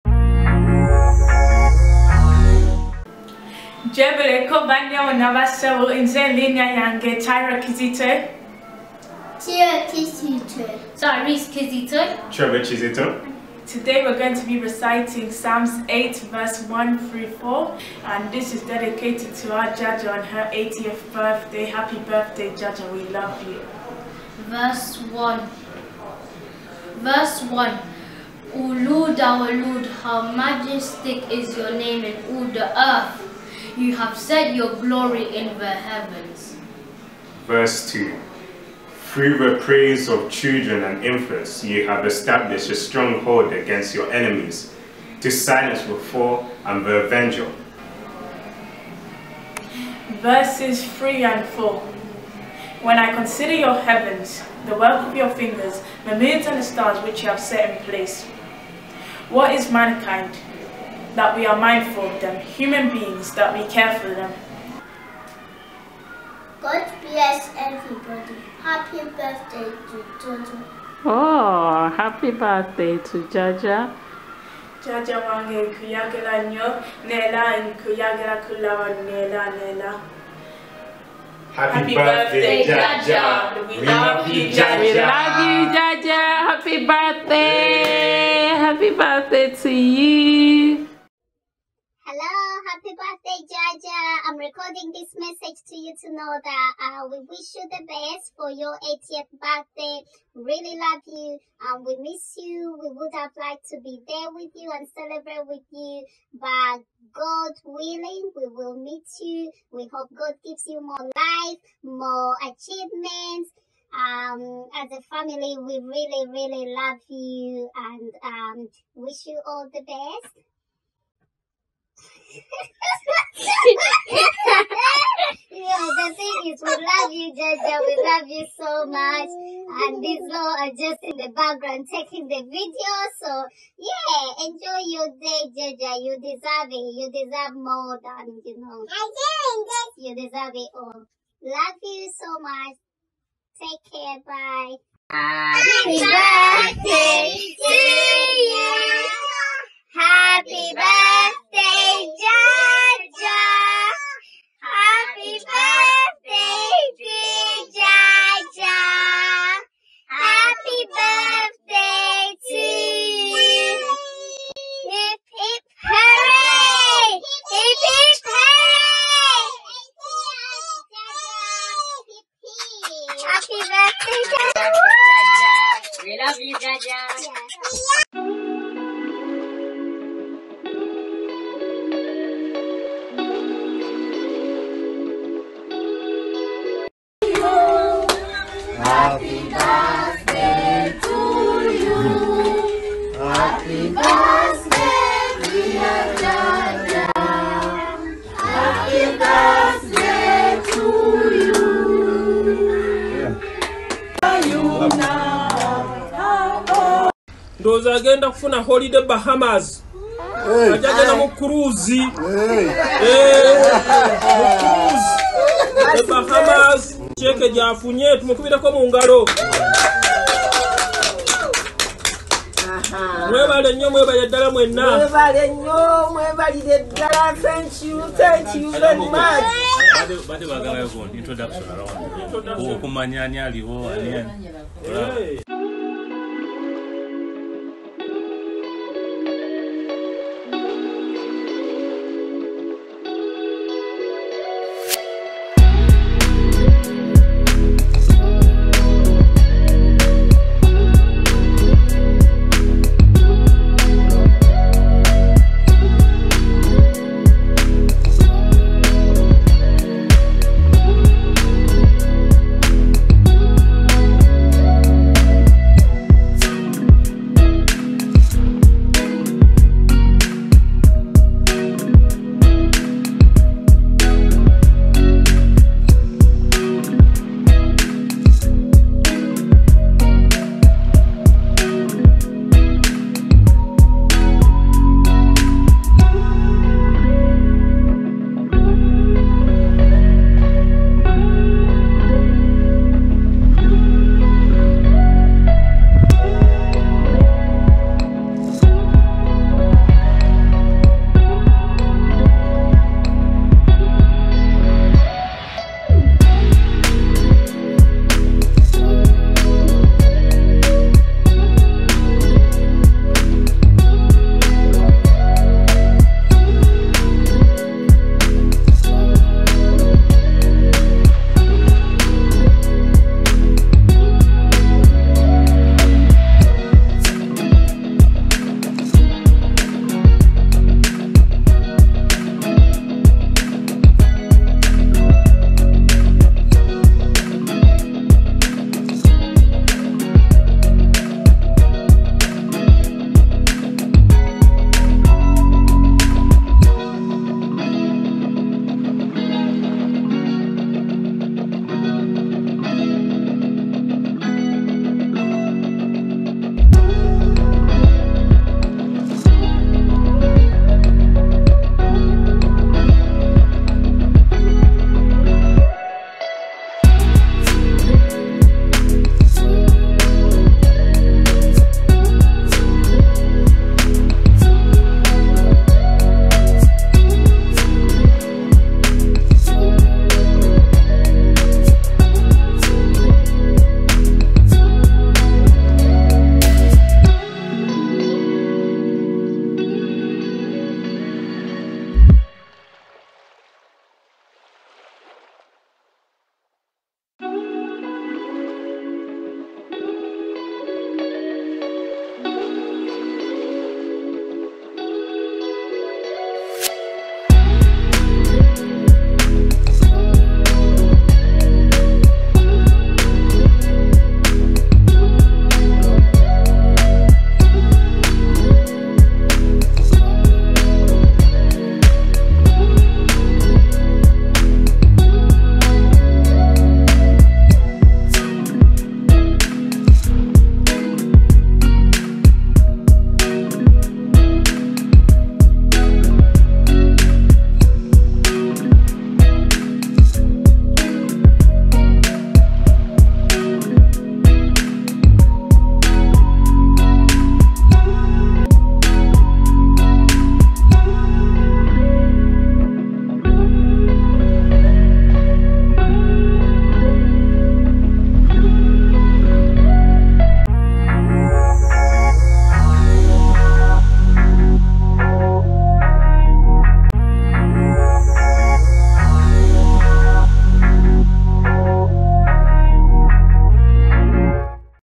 GRANDMA'S/ JAJJA’S 80TH BIRTHDAY SPEECH || SPILLING THE TEA ON SECRETS FOR LIVING A LONG LIFE !!!
Happy 80th Birthday once again dear Grandma/Jajja- WE LOVE YOU!!!

BIBLE VERSES (PSAM 8 VERSE 1-4)

1 Lord, our Lord, how majestic is your name in all the earth!  You have set your glory
    in the heavens.

  2 Through the praise of children and infants you have established a stronghold against your enemies, to silence the foe and the avenger.

   3 When I consider your heavens, the work of your fingers, the moon and the stars,
    which you have set in place,

4 what is mankind that you are mindful of them,
    human beings that you care for them?

 God bless everybody Happy Birthday Jajja !!!

FB/ Instagram: Claire's Klassy Makeovers